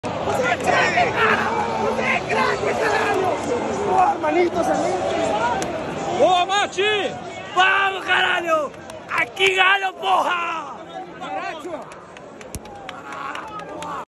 MATIAS ZARACHO: "AQUI É GALO, P$#@" 🖤🤍
Matias Zaracho comemorou ao estilo Ronaldinho no Mineirão após o título de pentacampeão mineiro